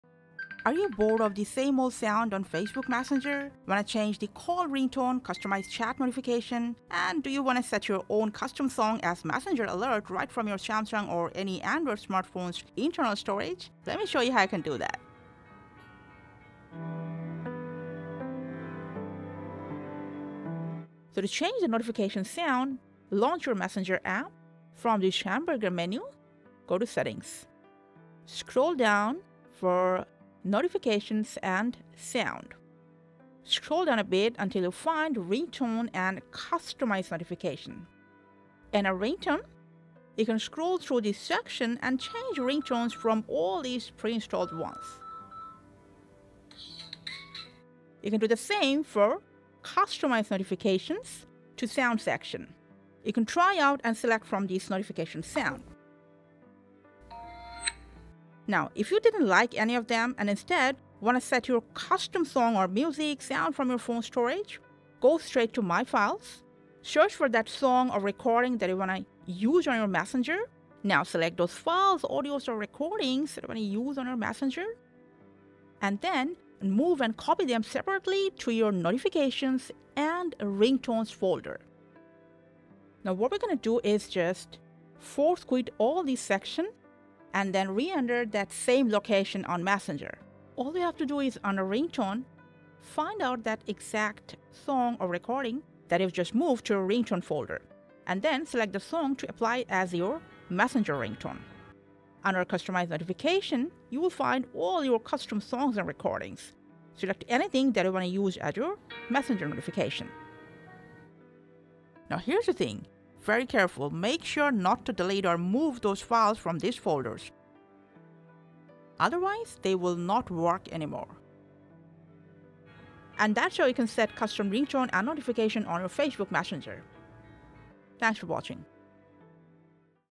How to Change Messenger Ringtone / Notification Sound! [Customize in Samsung Android]
Need help to change the messenger call ringtone and Chat notification sound on any Android Phone to set a customized tone? Searching for a way to apply custom songs for Ringtone and notification sounds on FB Messenger from Samsung Galaxy, Google Pixel, OnePlus, or any other Android Phone? Well, you’ve come to the right video.

In this video, the Fix369 Team will show you the step-by-step guide to:

* Change messenger Message tone and call ringtone on Android Phone.
* Set Custom songs for Facebook Messenger Ringtone and Notification Tone.

0:00 Customize Messenger Ringtone / Notification Sound on Android
0:20 Change Messenger Ringtone and Notification sound
0:58 Set Custom song as ringtone and Notification Sound on Messenger
1:51 Please Note